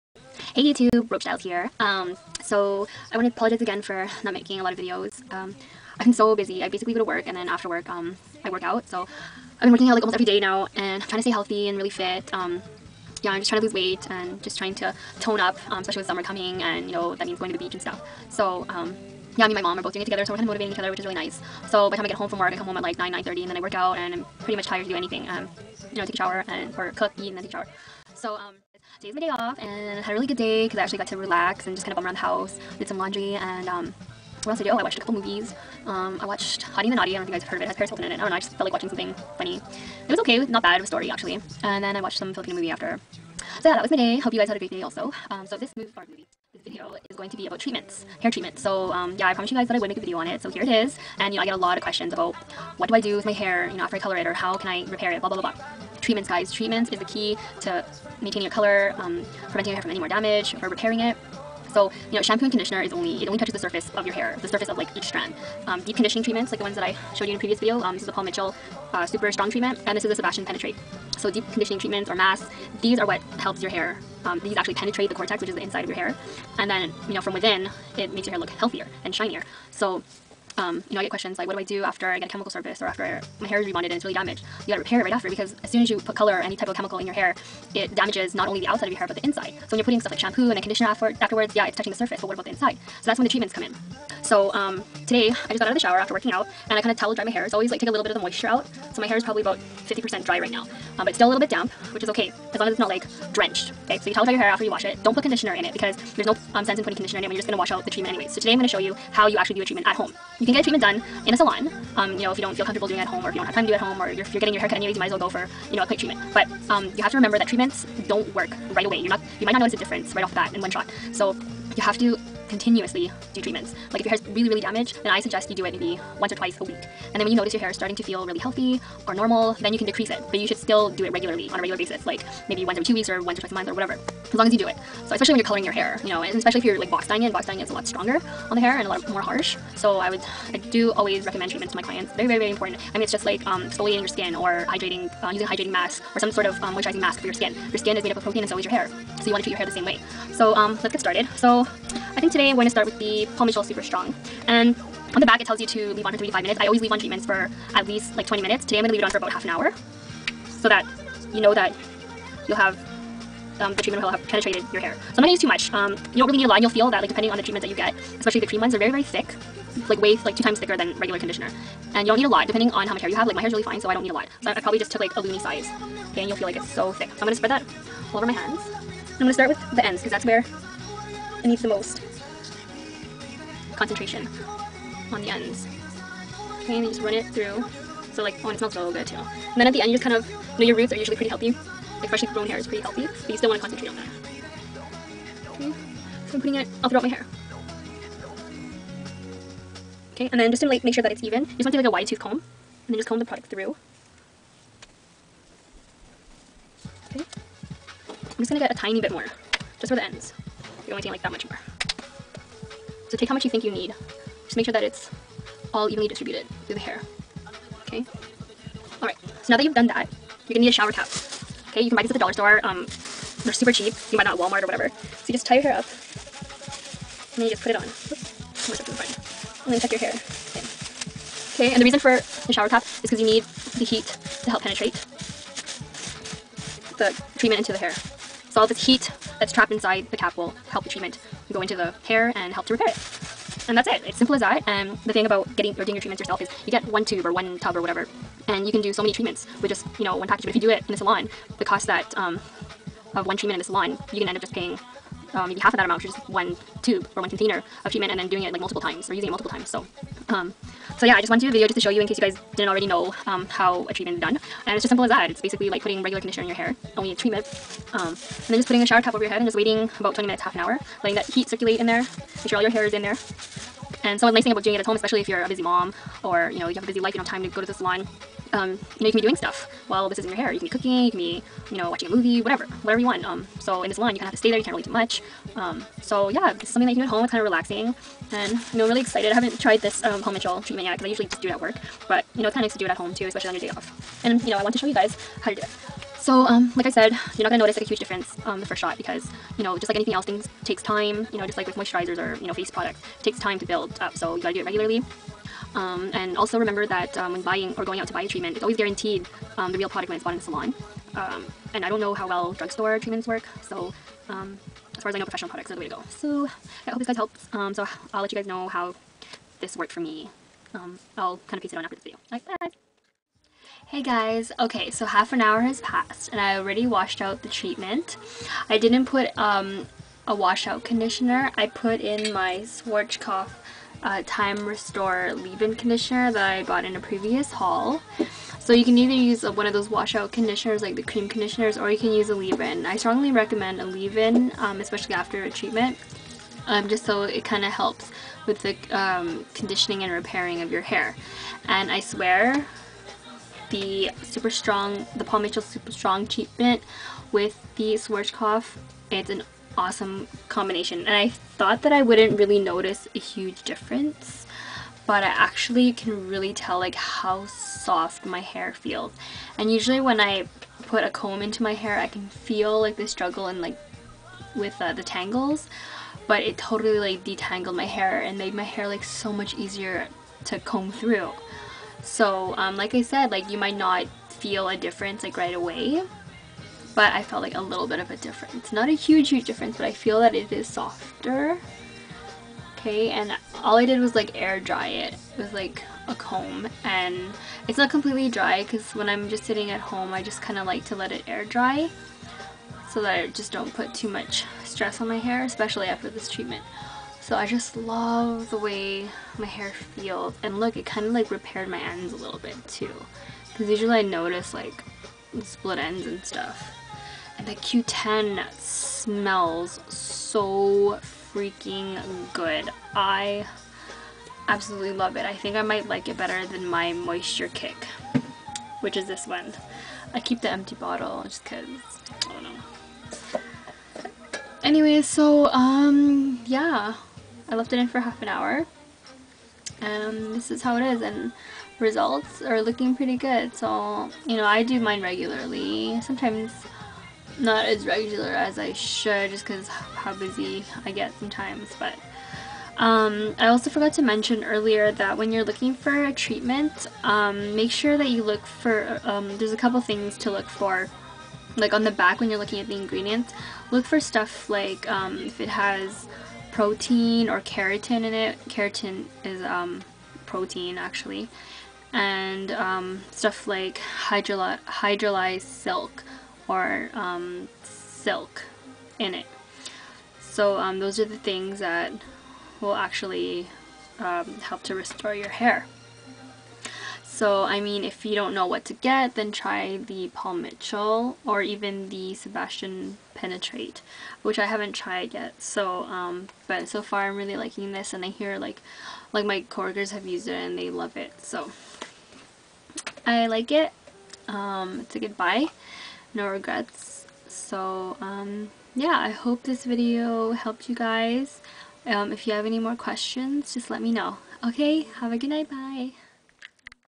Hair care: Treatment tutorial/advice/review
Here it is guys! the treatment video ive been talking about making for a while now.  Hope this vid kinda helps you guys out a bit.  If yo any further questions let me know!